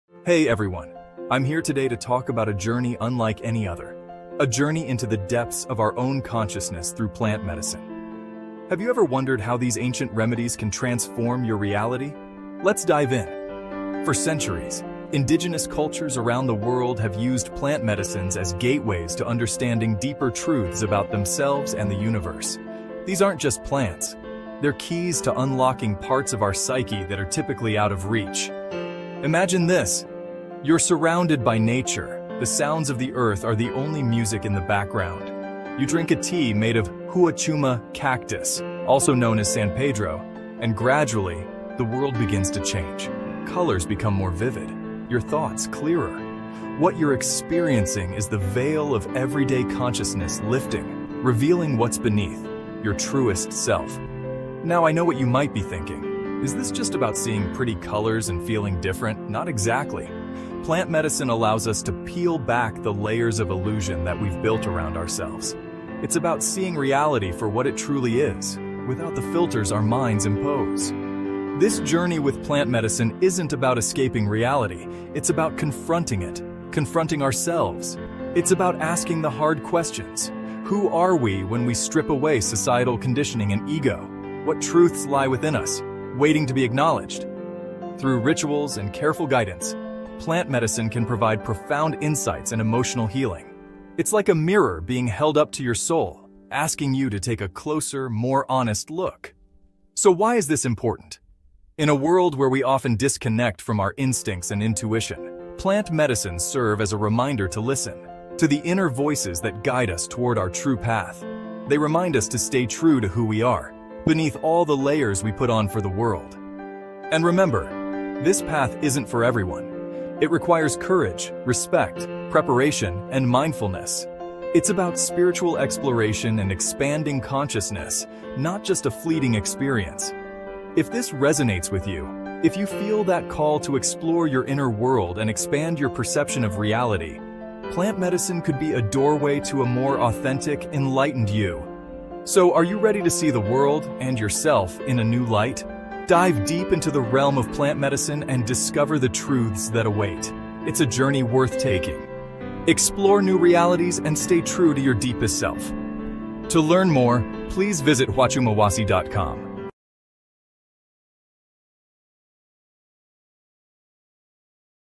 Huachuma, a.k.a. San Pedro Cactus: A Magical Journey Toward Your Authentic Self | Huachuma Wasi
Huachuma, a.k.a. San Pedro cactus is revered for its profound psychoactive, healing, and spiritual properties.

In traditional Andean shamanic practices, Huachuma is used for healing and divination. It allows communication with the spirit world and insight into personal or communal issues. It facilitates physical and mental healing, enhances sensory perception, creates a profound introspective experience and a personal transformation. The experiences often lead to increased self-awareness, emotional healing, and a stronger connection to nature and the universe.

The cactus is seen as a bridge between the human and divine realms.
Huachuma fosters a deep connection with the natural world. People who have taken Huachuma in a ceremonial setting often report a heightened appreciation for nature and a sense of unity with all living things.
In folk medicine, San Pedro is used to treat a variety of physical ailments, and nervous disorders. It is believed to have anti-inflammatory and antimicrobial properties.

Huachuma is a magical cactus, that opens the doors to higher consciousness.